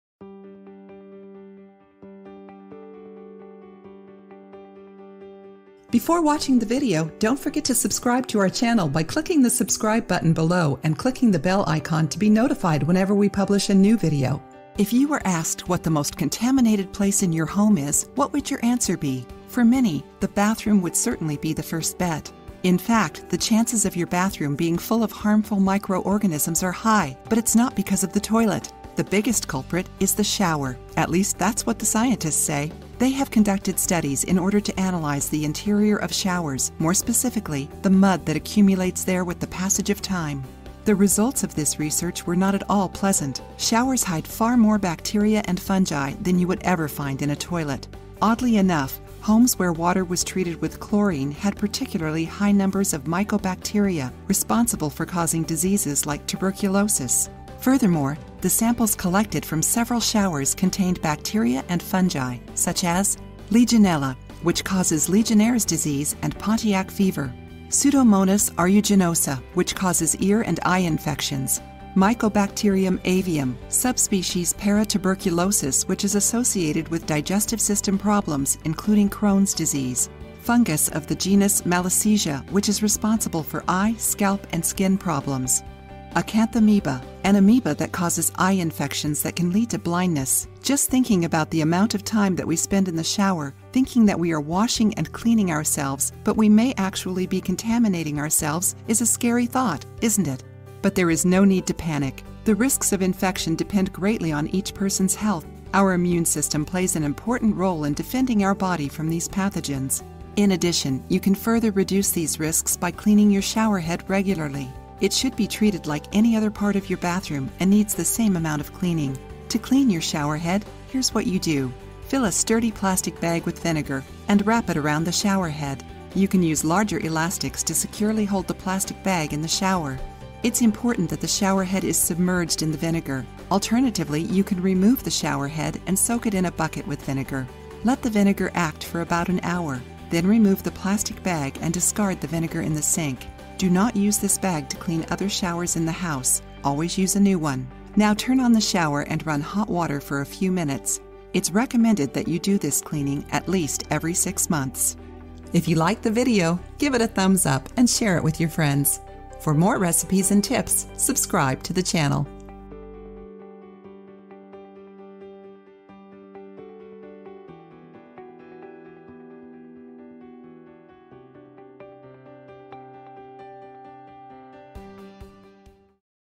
Dirty Shower Heads Could Be Deadly, Here's How To Clean Them!
Bacterial build-up on shower heads is associated with a variety of health problems. In this video, we will show you how to clean a shower head, inr order to protect your health.

If you were asked what the most contaminated place in your home is, what would your answer be?

For many, the bathroom would certainly be the first bet.

In fact, the chances of your bathroom being full of harmful microorganisms are high, but it’s not because of the toilet.

The biggest culprit is the shower - at least that’s what the scientists say.

Showers hide far more bacteria and fungi than you would ever find in a toilet.

Just thinking about the amount  of time that we spend in the shower, thinking that we are washing and cleaning ourselves but we may actually be contaminating ourselves, is a scary thought, isn’t it?

But there is no need to panic.

The risks of infection depend greatly on each person’s health.

In addition, you can further reduce these risks by cleaning your shower head regularly.

It’s recommended that you do this cleaning at least every 6 months.